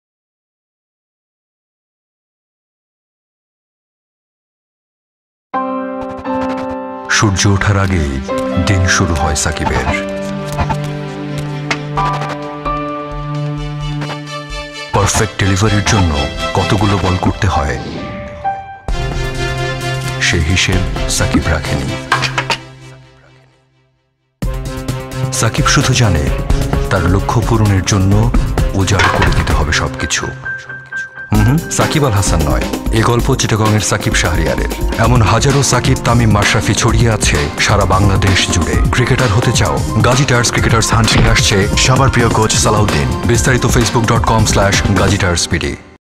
Gazi Tyres Cricketers Hunt (TVC-2)
Campaign : Gazi Tyres Cricketers Hunt (TVC-2)
Duration : 40 Sec

Client : Gazi Tyres

Agency : DOTCREAT

Director : Partha Pratim Nath
Production House : Cinema Cafe